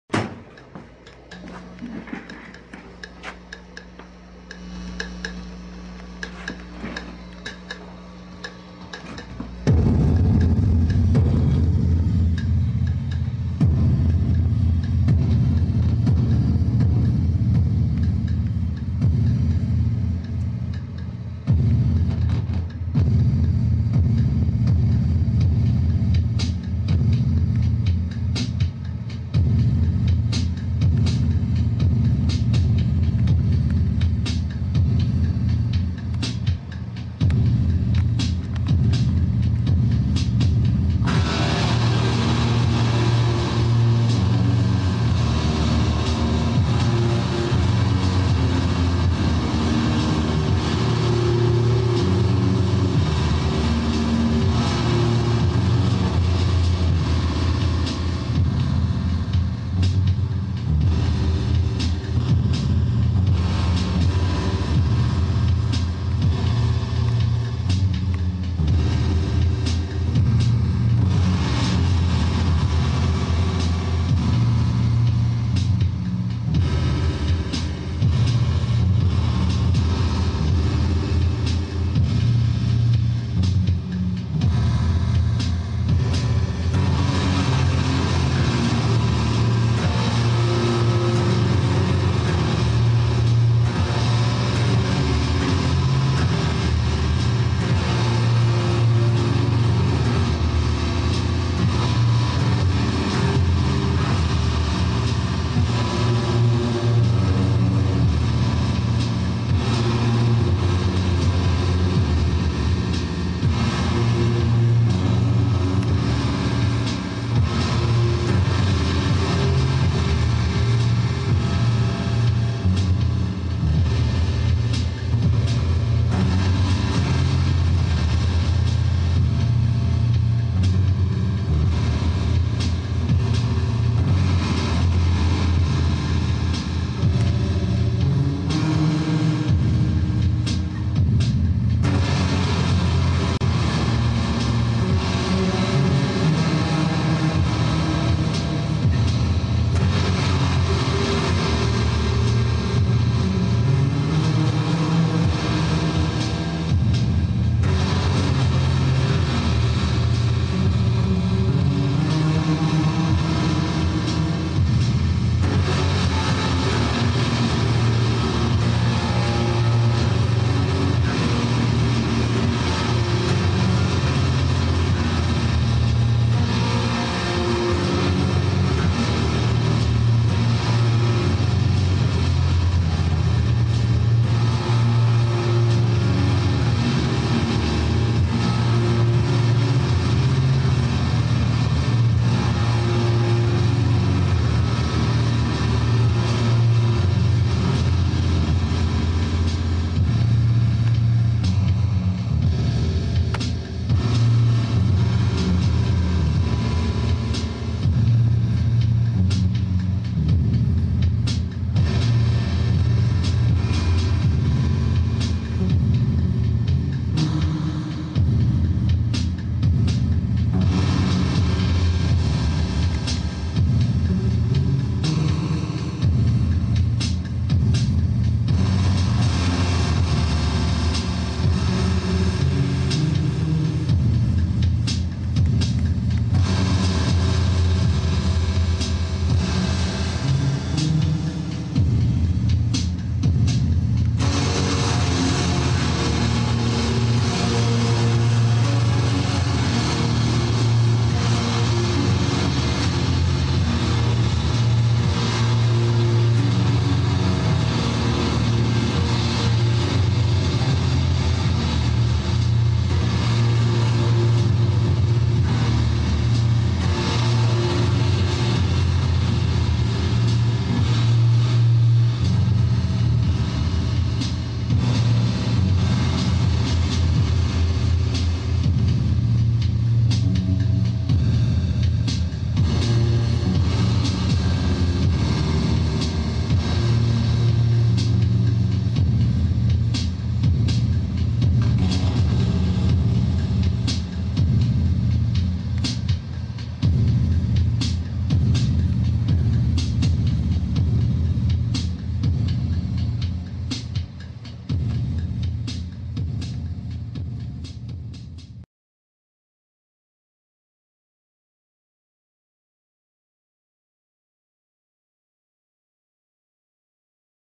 Negative
Again Brendan Lough and myself jamming.
Brendan on the guitar and i was using Korg emx 1.
So i thought i would put together a video with footage i had.